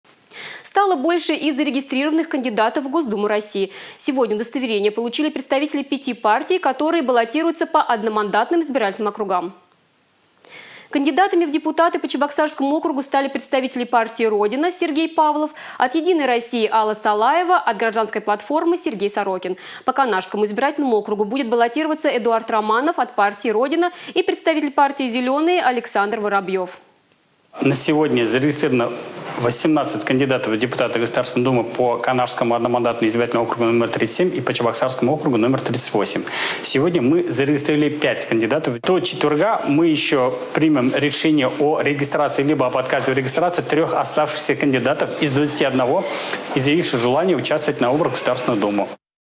Зарегистрированных кандидатов в Госдуму России стало больше
Стало больше и зарегистрированных кандидатов в Госдуму России. Сегодня удостоверения получили представители пяти партий, которые баллотируются по одномандатным избирательным округам.